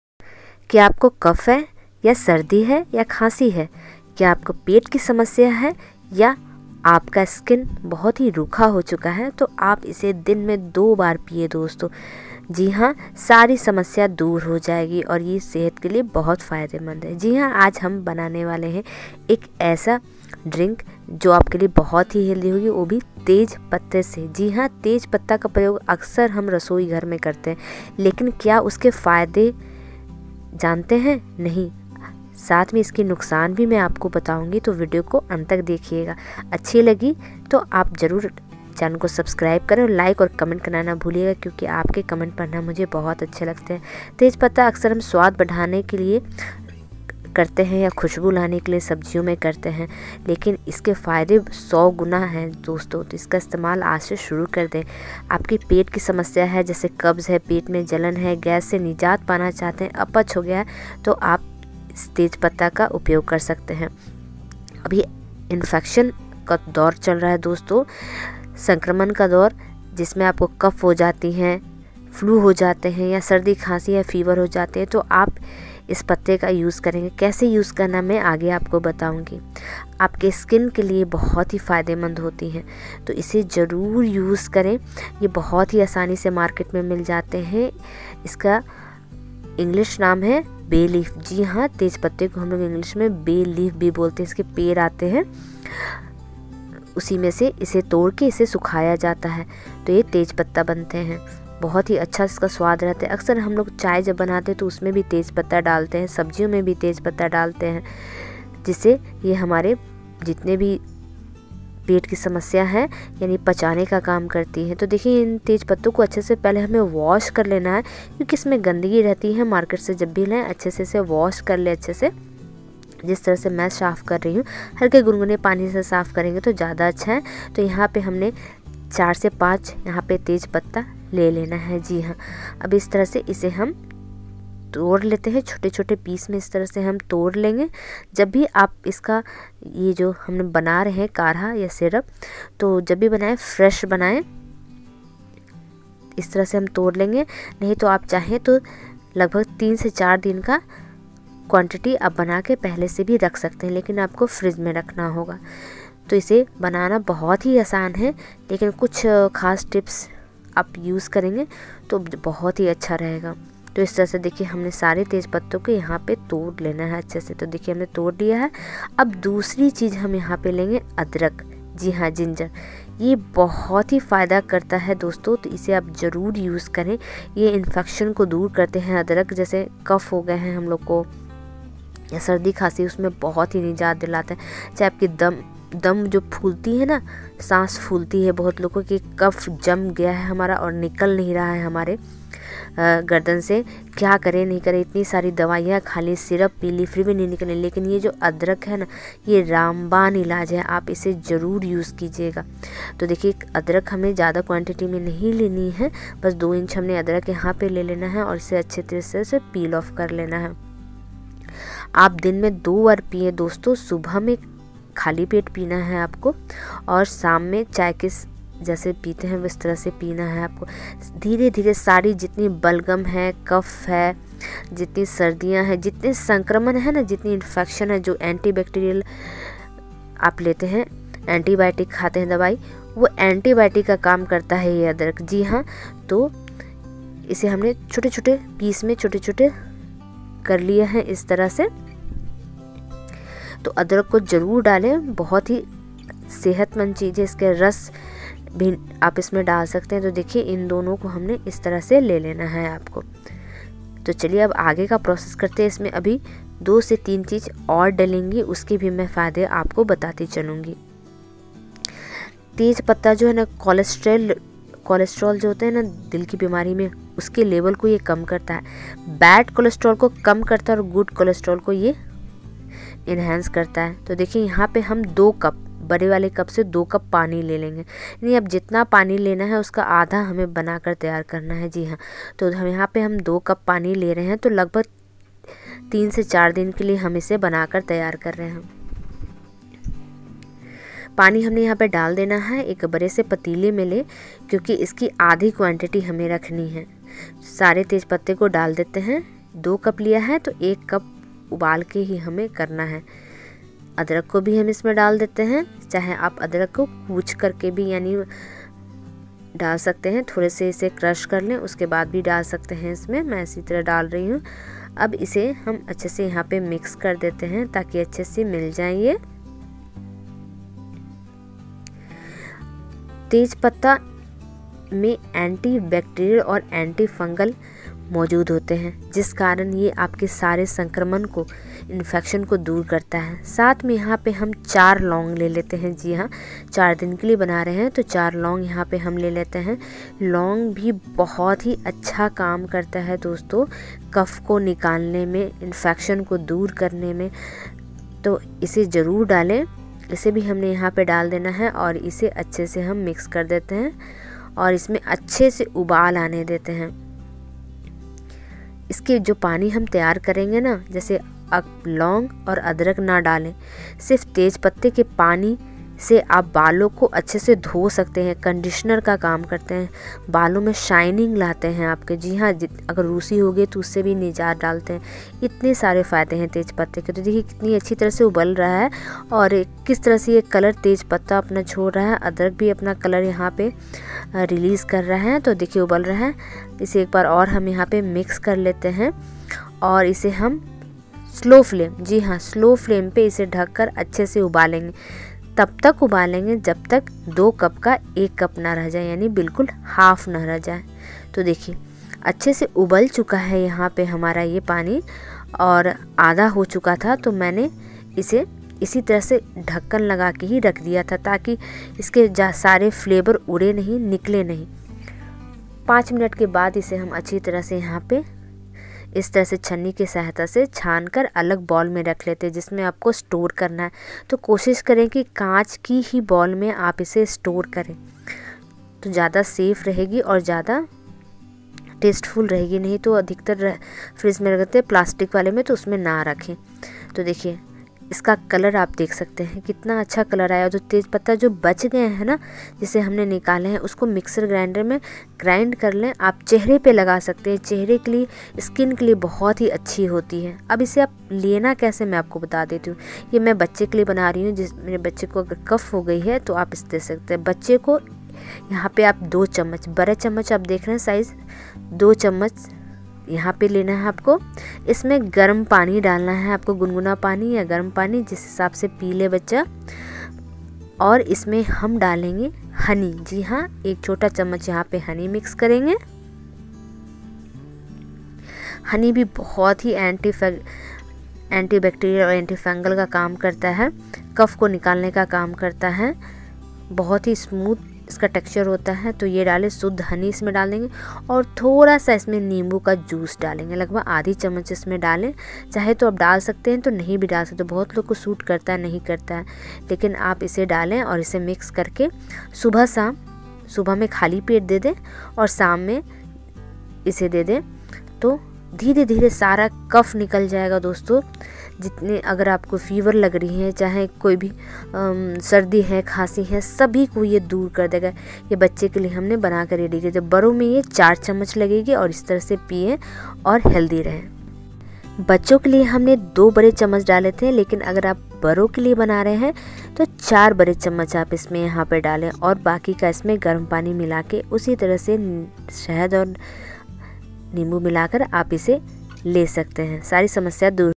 बस दो चम्मच पीले फेफरे छाती गले में सालो से जमा हुआ कफ बलगम तुरंत बाहर निकलें।Home remedy coughcold
बस दो चम्मच पीले फेफरे छाती गले में सालो से जमा हुआ कफ बलगम तुरंत बाहर
 निकलें,दोबारा कभी नहीं होगा